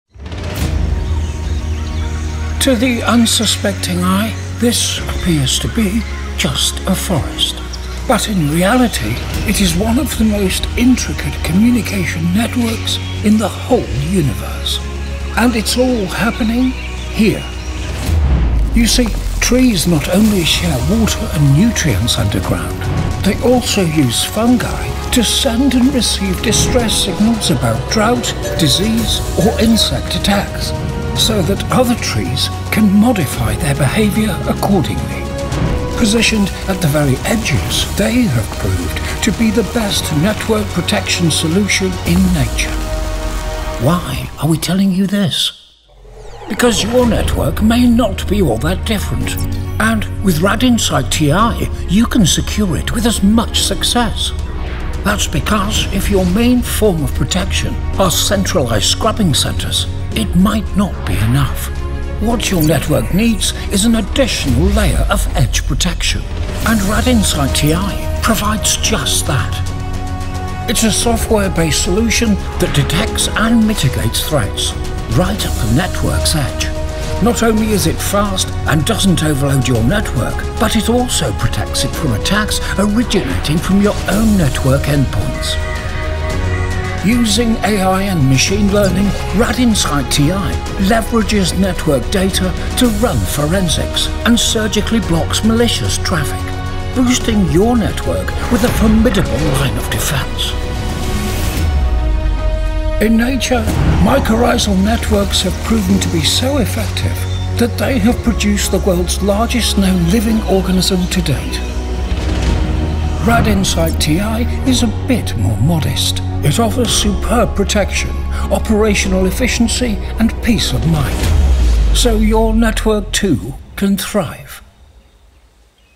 Threat Intelligence at the Edge with RADinsight TI
Network-embedded security that is implemented at the edge! Discover the power of RADinsight TI.

To the unsuspecting eye, this appears to be… just a forest. But in reality, it is one of the most intricate communication networks in the whole universe.

And it’s all happening...here. You see, trees not only share water and nutrients underground. They also use fungi to send and receive distress signals about drought, disease, or insect attacks, so that other trees can modify their behavior accordingly.

Scientists call these fungi-based communication systems- mycorrhizal networks.

Positioned at the very edges, they have proved to be the best network protection solution in nature.

Why are we telling you this? Because your network, may not be all that different. And with RADinsight TI, you can secure it with as much success. That’s because if your main form of protection is centralized scrubbing centers –  it might not be enough.

What your network needs is an additional layer of edge protection. And RADInsight TI - provides just that.

It’s a software-based solution that detects and mitigates threats right at the network edge. Not only is it fast and doesn’t overload your network, it also protects you from attacks originated by your own network endpoints.

Using unique AI and machine learning, RADInsight TI leverages network data to run forensics and surgically blocks malicious traffic, boosting your network with a formidable line of defense.

In nature, Mycorrhizal networks have proven to be so effective that they have produced the world’s largest known living organism to date:

RAD Insight TI is a bit more modest:
It offers superb protection, operational efficiency, and peace of mind – so your network too, can thrive.